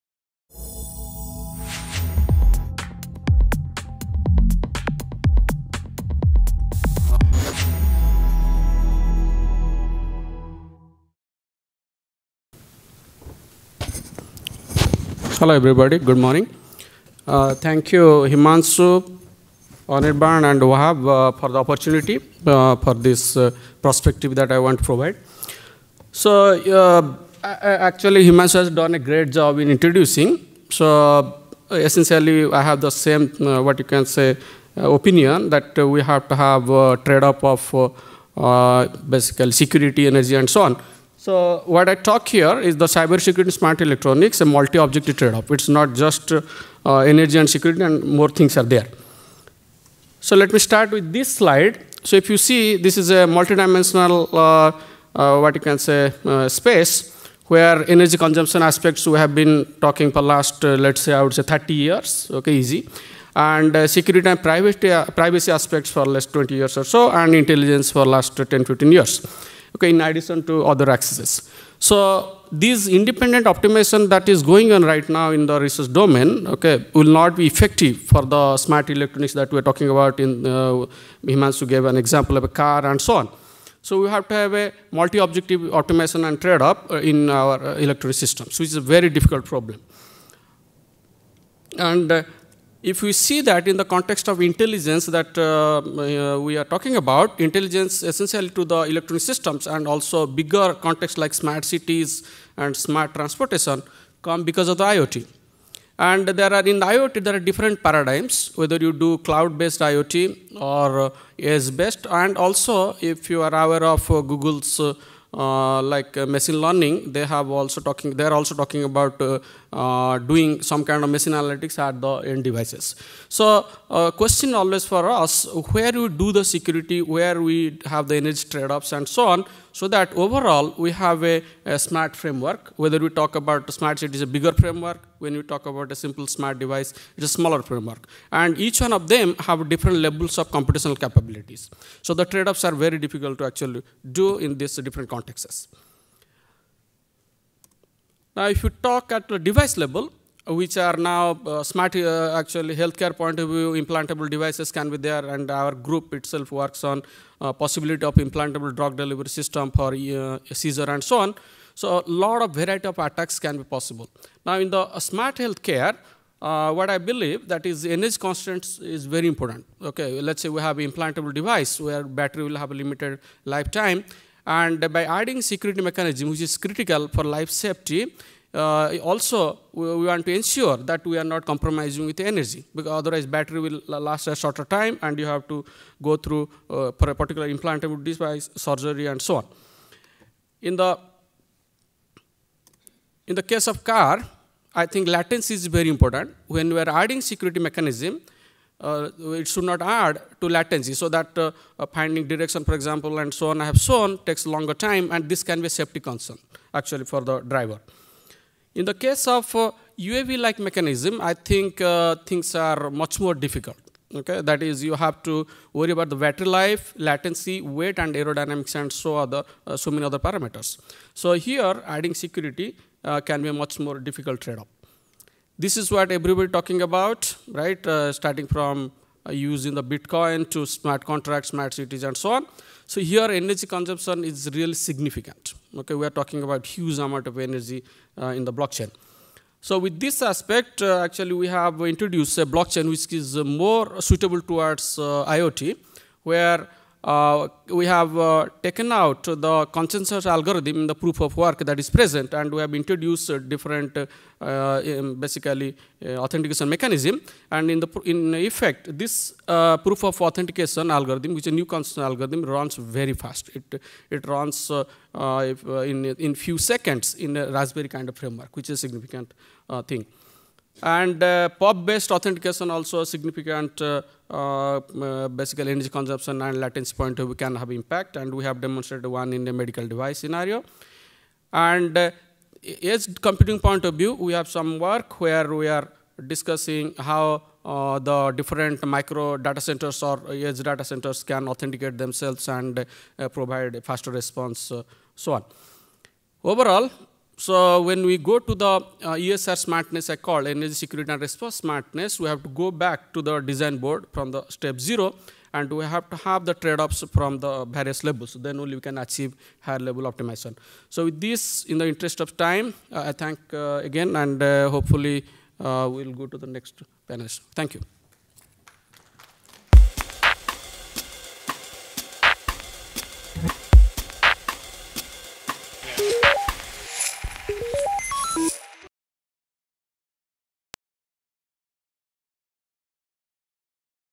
Saraju P. Mohanty. Cybersecurity in Smart Electronics – A Multi-Objective Trade-off
Cybersecurity in Smart Electronics – A Multi-Objective Trade-off
Saraju P. Mohanty
University of North Texas, USA

ICCE 2019
37th IEEE INTERNATIONAL CONFERENCE ON CONSUMER ELECTRONICS (ICCE)
January 11-13, 2019, Las Vegas, USA